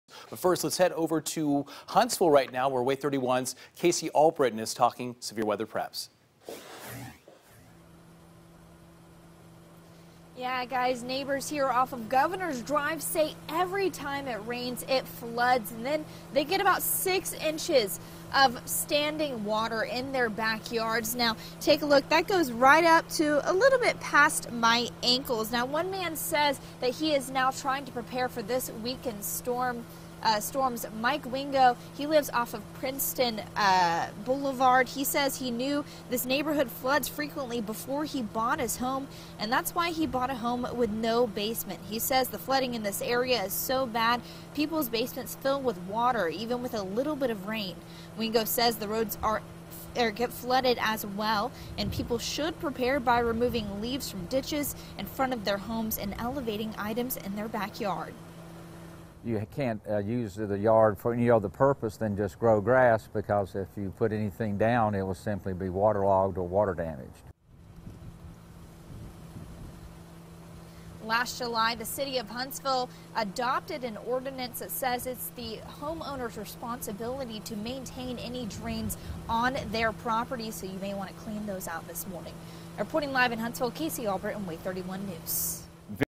Flooding Preparation
Homeowners off of Governors Dr. say they can have up to 6 inches of standing water in their yards.